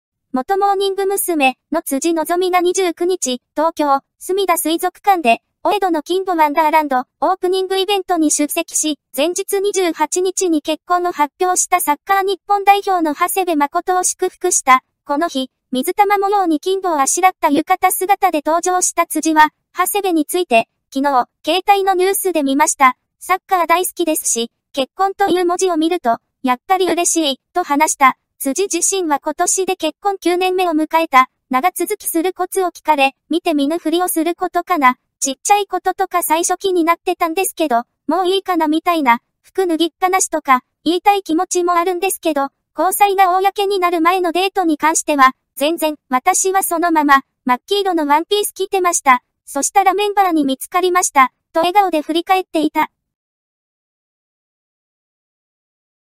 辻希美 結婚発表した長谷部誠を祝福
辻希美 結婚発表した長谷部誠を祝福　動画で解説しています。

 最新芸能関係の暴露ネタ 極秘情報を動画にて発信をしています。

（関連動画）
辻希美 『辻ちゃんが作った肉じゃがみたいなやつ本当に美味いのかなぁ～？罪』で確保!?ｗｗｗ【ブログ刑事】